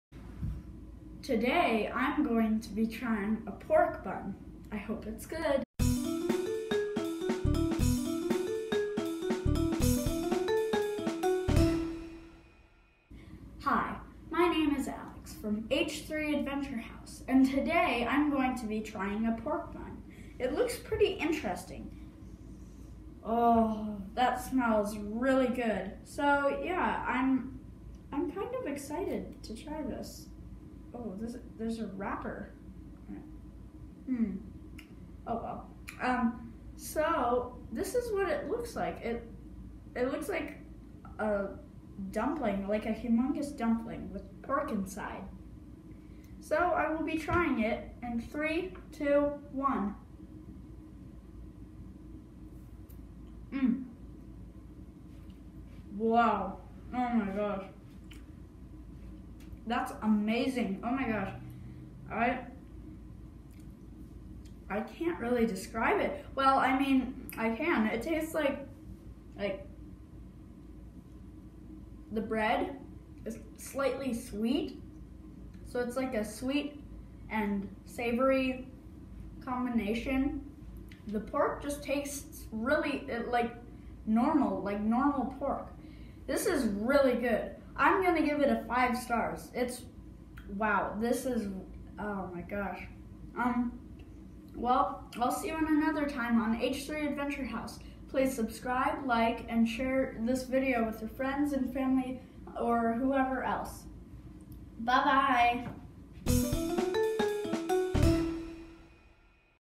H3 Advendture House: pork bun
Today Alex tries a fresh pork bun from our favorite Asian market.

Adventure House: "You don't have to like the food, but you do have to try it."